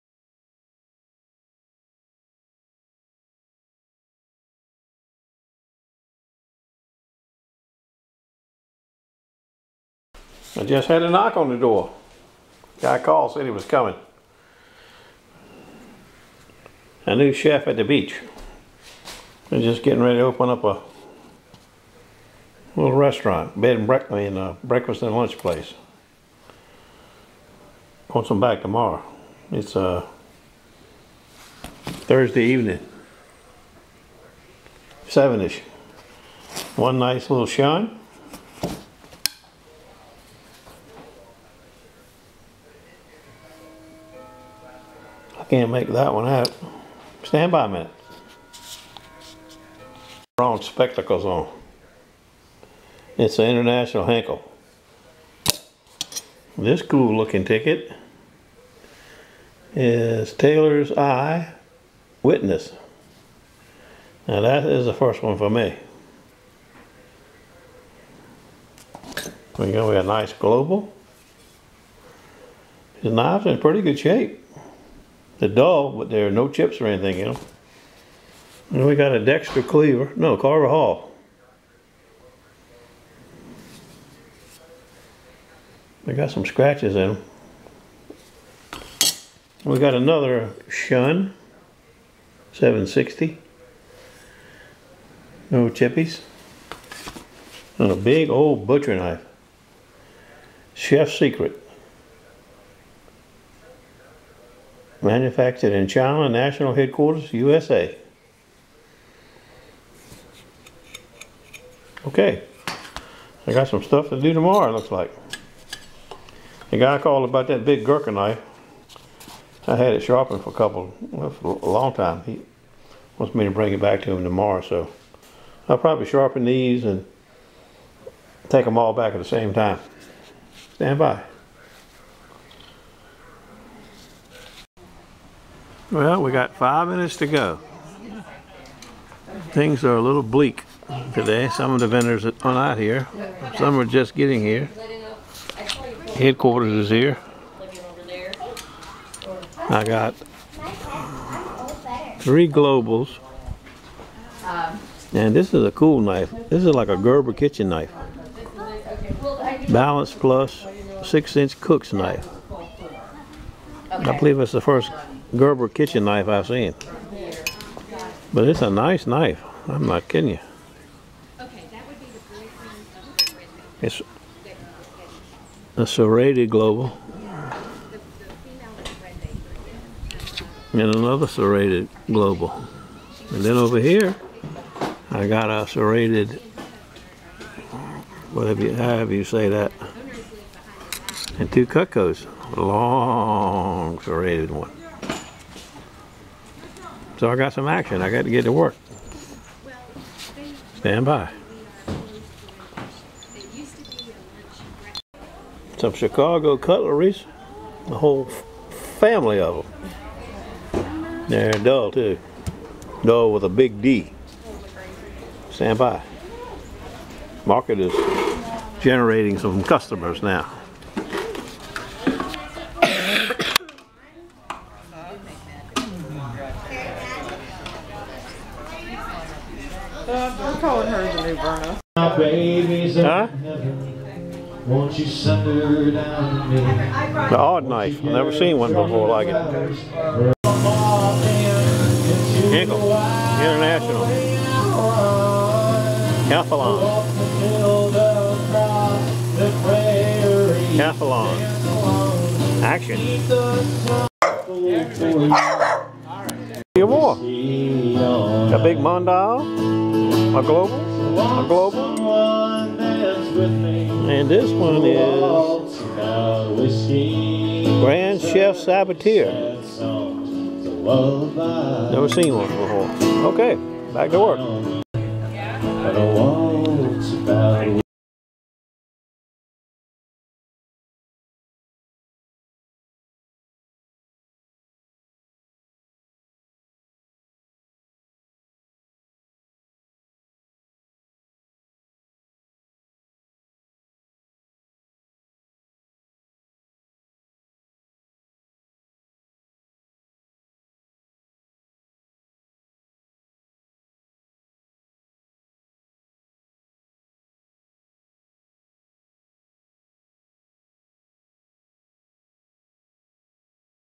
OCT 24th - Farmers Fare Market at East Beach in ocean view
Stop by for a visit – say hello
East Beach Farmers Fare today was yet another good sharpening day today – maybe a bit slower, but good day for me – market was a bit slow and a few less vendors.
Going home to unload the truck and head north to Nokesville, va and a dog rescue.  Jack is getting a new friend to keep him from being so lonely.  Stand by for that adventure. Oct. 24th.
see vid from Oct. 25th.